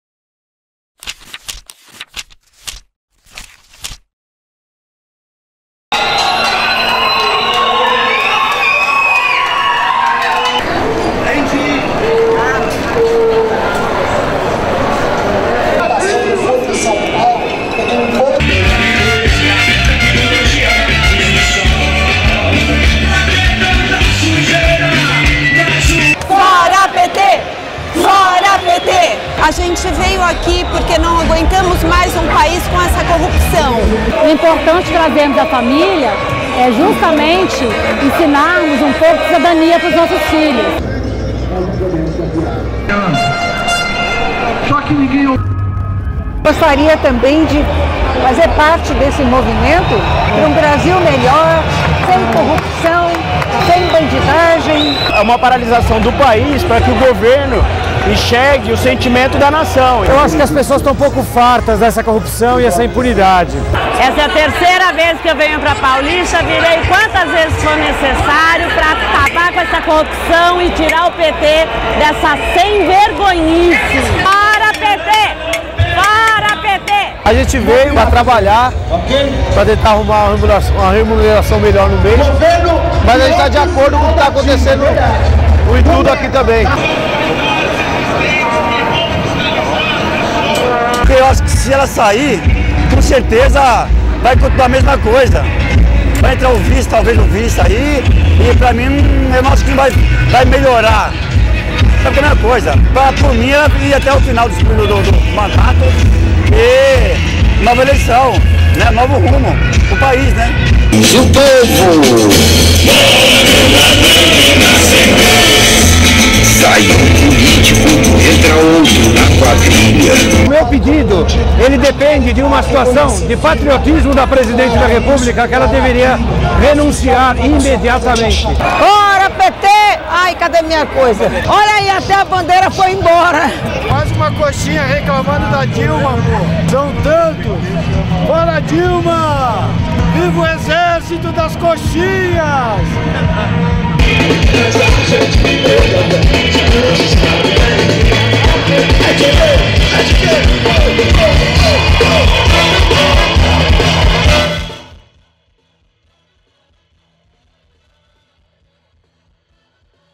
Protesto com jeitão de festa na Paulista
Com bandeiras do Brasil, apitos e vuvuzelas, quem foi a Paulista neste dia 16 pedia pelo fim da corrupção, enquanto bebericava cervejas e levava o cão para passear.